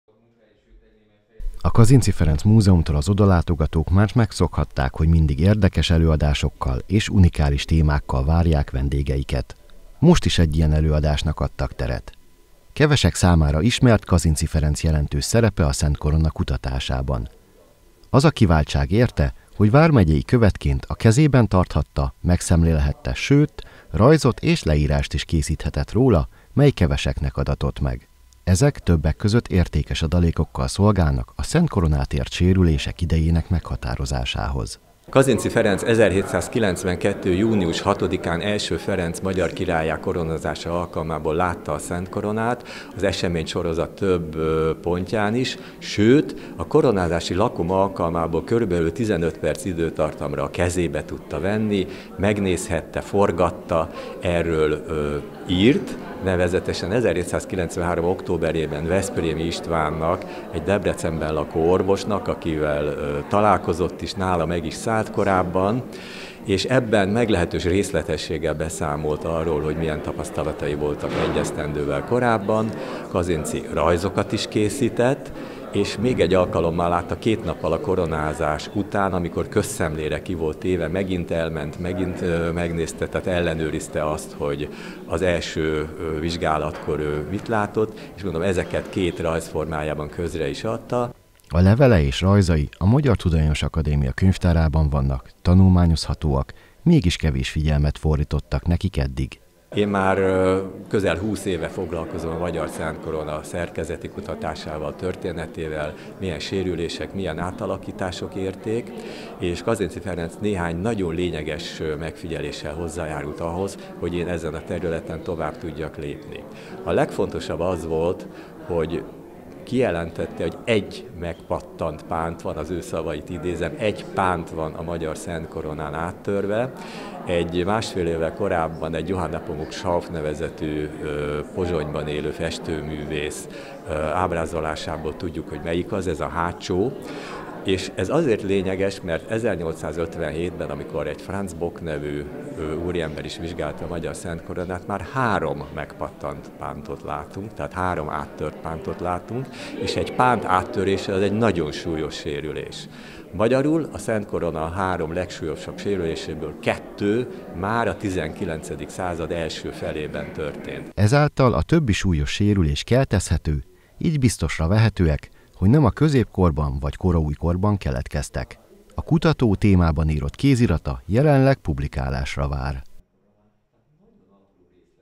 „Kazinczy Ferenc szerepe a Szent Korona kutatásában” címmel tartottak előadást Sátoraljaújhelyen
A sátoraljaújhelyi Kazinczy Ferenc Múzeumban „Kazinczy Ferenc szerepe a Szent Korona kutatásában” címmel tartott előadást Németh Zsolt, a Magyarságkutató Intézet Eszmetörténeti Kutatóközpontjának tudományos főmunkatársa. Nyelvújítónk vármegyei követként közelről megvizsgálhatta a koronát a 18. század végén, melyről részletes leírást és vázlatokat is készített.